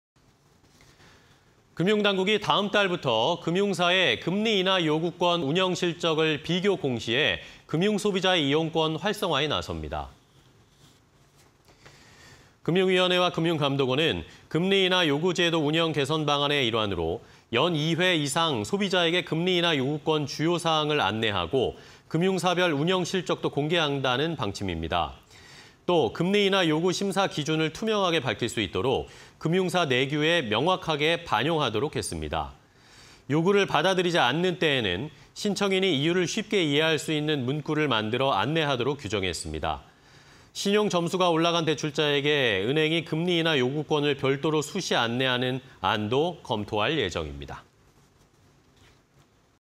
내달부터 금융사 '금리인하요구권 운영 실적' 비교 공시 (20220718)
금융당국이 다음 달부터 금융사의 금리인하요구권 운영 실적을 비교 공시해 금융 소비자의 이용권 활성화에 나섭니다.
금융위원회와 금융감독원은 금리 인하 요구제도 운영 개선 방안의 일환으로 연 2회 이상 소비자에게 금리 인하 요구권 주요 사항을 안내하고, 금융사별 운영 실적도 공개한다는 방침입니다.
또 금리 인하 요구 심사 기준을 투명하게 밝힐 수 있도록 금융사 내규에 명확하게 반영하도록 했습니다.
요구를 받아들이지 않는 때에는 신청인이 이유를 쉽게 이해할 수 있는 문구를 만들어 안내하도록 규정했습니다.
신용 점수가 올라간 대출자에게 은행이 금리인하요구권을 별도로 수시 안내하는 안 등도 검토할 예정입니다.